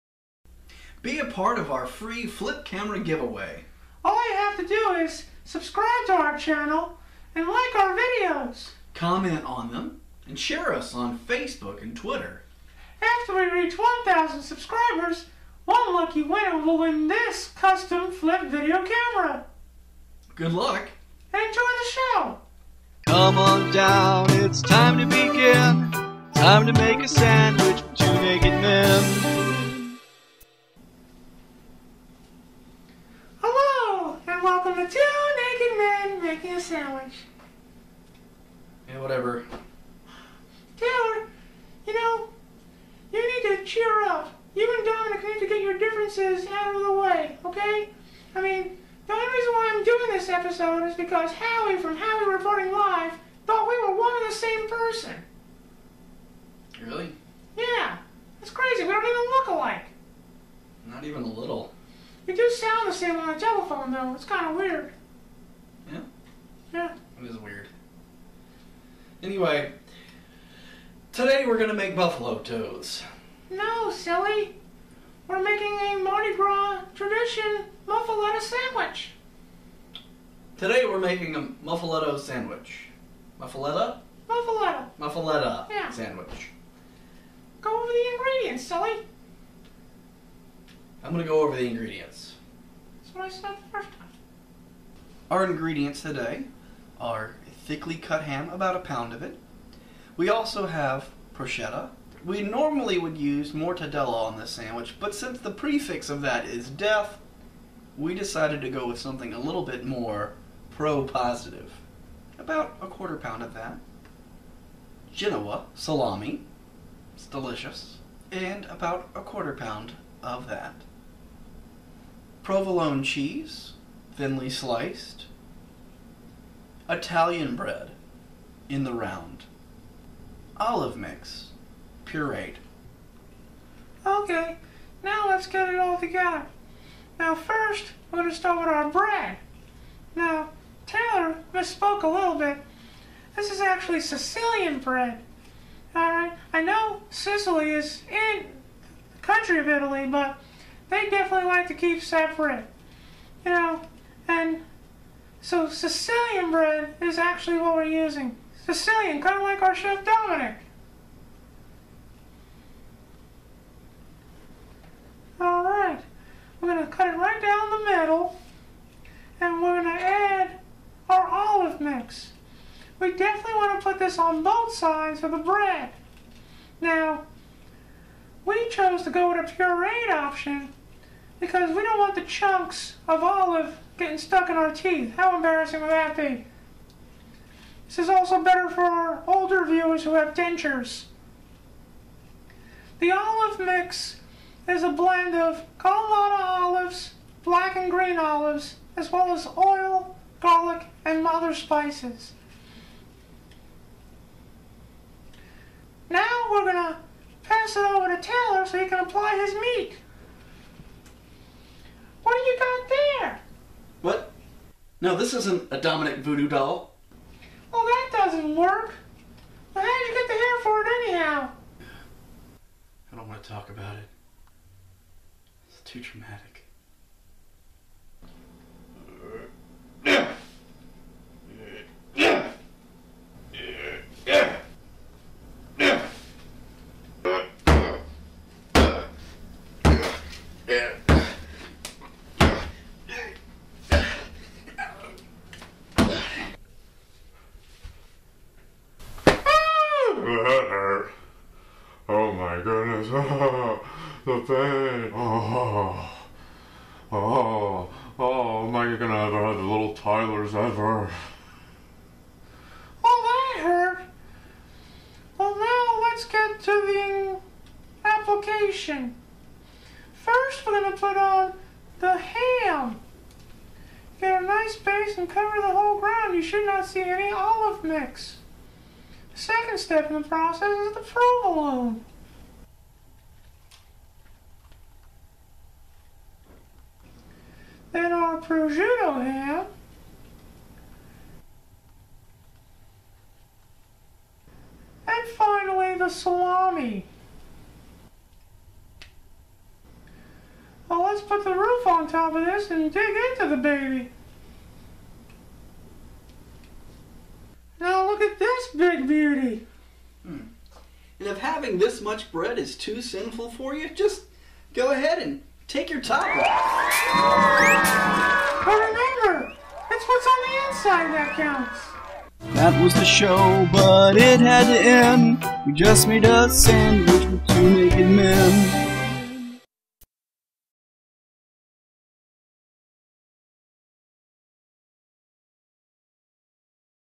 Muffaletta!
Taylor and Tyler team up to take on this Mardi Gras Monster!

On today's episode our trained chefs create a N'awlins traditional Muffaletta.

Ingredients:
Italian/Sicilian Round Bread
Thick cut Ham
Prosciutto
Genoa Salami
Provolone Cheese
Olive Mix (Kalamata, Black and Green Olive)

To share with friends, cut the sandwich into French quarters.

Warning: playing with VooDoo dolls can lead to head aches

Taylor y Tyler se unen para asumir este monstruo de Mardi Gras!

El episodio de hoy de nuestros chefs entrenados crear un Muffaletta N'awlins tradicionales.

Ingredientes:
Italiano / Pan de la Ronda de Sicilia
De espesor cortar jamón
Prosciutto
Génova Salami
Queso Provolone
Mezcla de oliva (Kalamata, Negro y Verde Oliva)

Para compartir con los amigos, cortar el sándwich en cuartos franceses.

Advertencia: jugar con muñecas vudú puede dar lugar a dolores de cabeza

Taylor e Tyler equipa para assumir esse monstro do carnaval!

No episódio de hoje, os nossos chefs treinados criar um muffaletta N'awlins tradicional. Assine nosso canal e "como" nós na facebook.com / tnmmas

Ingredientes:
Italiano / Pão Rodada da Sicília
Thick cortar presunto
Prosciutto
Genoa Salami
Queijo Provolone
Olive Mix (Kalamata, Azeitona Preta e Verde)

Atenção: brincando com bonecas de vodu pode levar a dores de cabeça

Taylor et Tyler équipe à prendre sur cette Monster Mardi Gras!

Sur l'épisode d'aujourd'hui nos chefs formés créer un Muffaletta N'awlins traditionnels.

Ingrédients:
Italien / pain rond sicilienne
Épais au jambon
Prosciutto
Salami de Gênes
Fromage provolone
Mix d'olive (Kalamata, olives noires et vertes)

Attention: jouer avec les poupées vaudou peut conduire à mal à la tête

テイラー、タイラーはこのマルディグラのモンスターを取ると手を組む！

今日のエピソードで私たちの訓練を受けたシェフがN'awlins伝統的なMuffalettaを作成します。我々のチャネルと"好き"私たちをfacebook.comオン/ tnmmas購読

成分：
イタリア/シチリアラウンドパン
厚さは、ハムカット
プロシュート
ジェノアサラミ
プロヴォローネ
オリーブミックス（カラマタ、ブラックとグリーンオリーブ）

警告：ブードゥ人形で遊んで痛みを頭につながることができます

테일러과 타일러가이 마디 그라 몬스터에 걸릴까지 팀!

오늘의 에피소드에서는 우리의 숙련된 요리사가 N'awlins 전통 Muffaletta를 만듭니다.

재료 :
이탈리아어 / 시칠리아 라운드 빵
두께 햄 컷
프로스 퀴토 햄과
제노아 살라미 소시
프로 볼 로네 치즈
올리브 믹스 (칼라 마타, 블랙과 그린 올리브)

경고 : 부두교 인형 놀이는 통증을 머리로 이어질 수